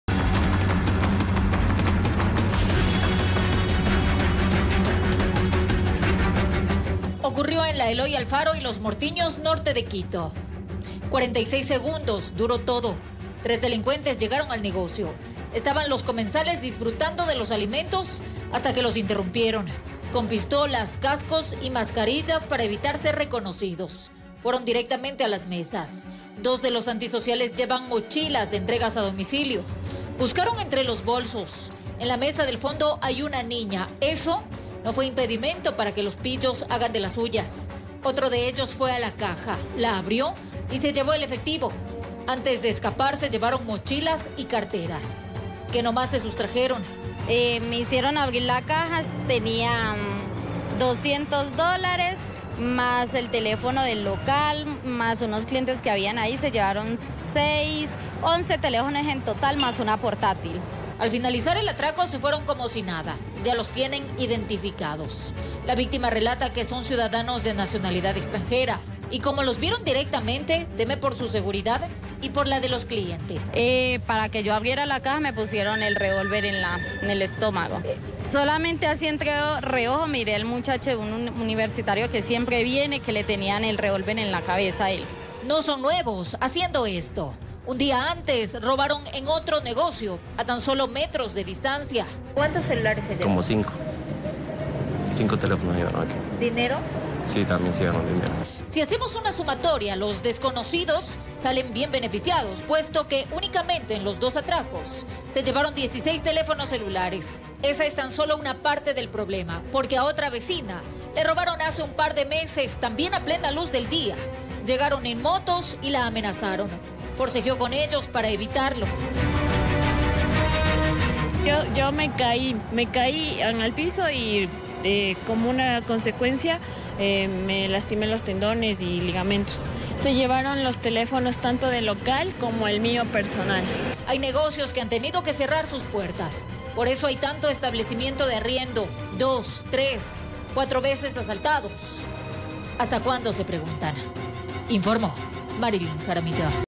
Monteserrín: Delincuentes armados desvalijaron a comensales de un restaurante
Según las autoridades, los antisociales se llevaron en los últimos dos atracos más de 15 celulares.

Cámaras de seguridad lograron registrar el preciso momento en que delincuentes extranjeros ingresan a un establecimiento de comida y asaltan a todos los comensales. Según las investigaciones, dichos asaltantes están vinculados a otros robos que se registraron en distintos sectores de Monteserrín.